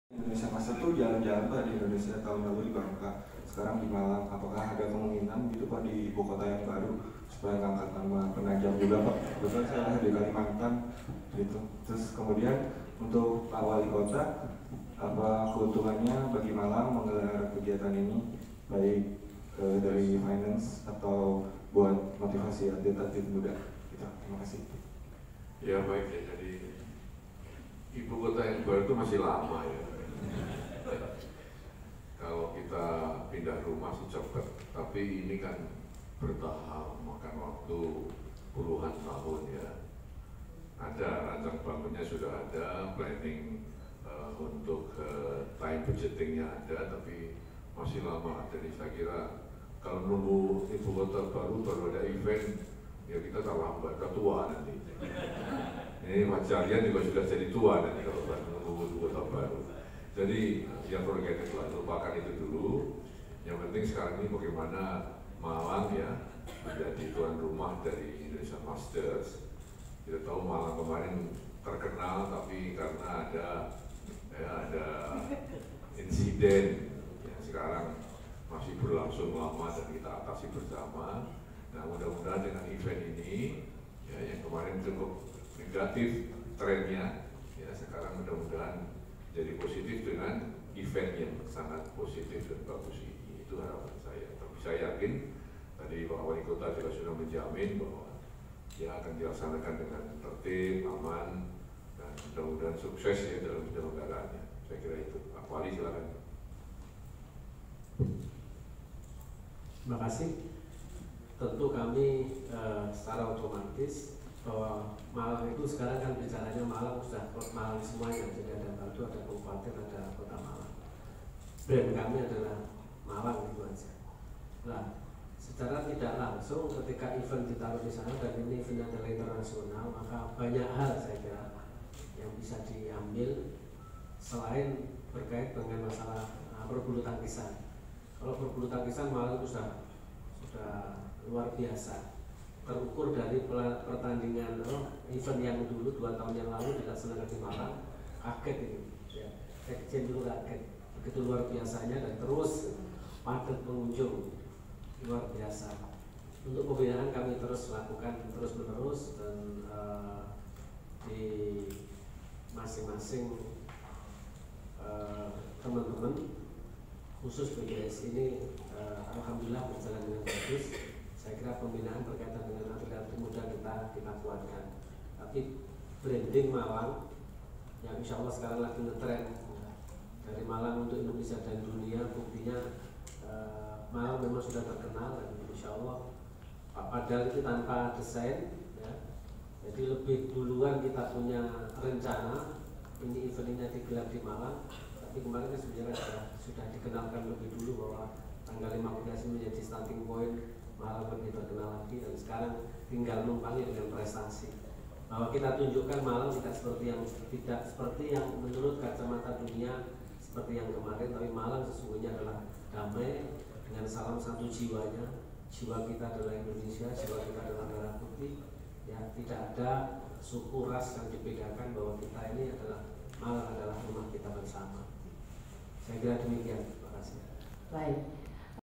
Wiranto : Kalau Nunggu Ibukota Baru, Fajar/Rian Keburu Tua
Prescon Yuzu Indonesia Masters Super 100 di Ritz Carlton, Jakarta, 11 September 2019